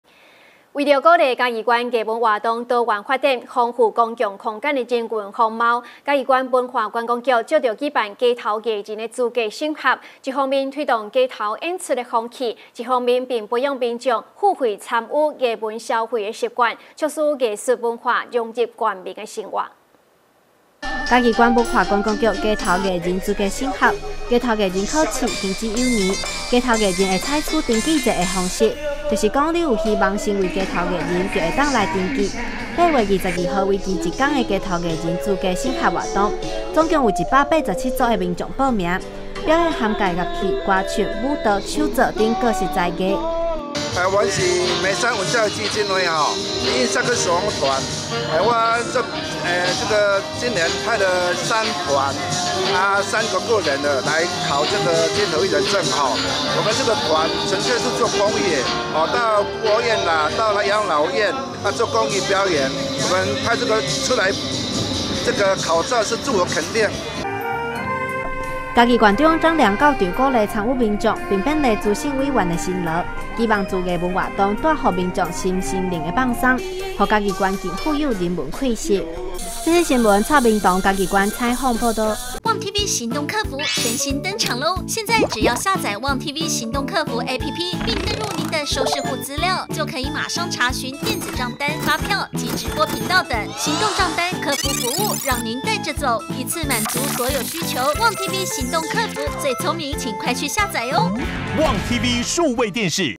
世新新聞 嘉義縣街嘉義縣頭藝人資審 187組才藝大展
109年8月24日 (一) 世新新聞

嘉義縣街嘉義縣頭藝人資審 187組才藝大展

為鼓勵嘉義縣藝文活動多元發展，豐富公共空間人文風貌，嘉義縣文化觀光局藉辦理街頭藝人資格審合，一方面推動街頭演出風氣，賦予藝人正當性與認同感外
一方面並培養民眾付費參與藝文消費習慣，促使藝術文化融入縣民的生活。